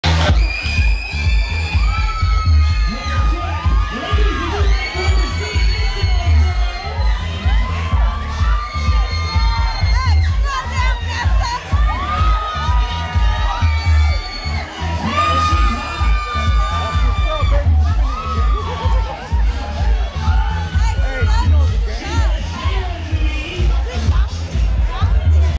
TWSDGRLE FASHION SHOW
NESSA WORKING THE RUNWAY AT KRESS IN HOLLYWOOD FOR TWSDGRLE CLOTHING LINE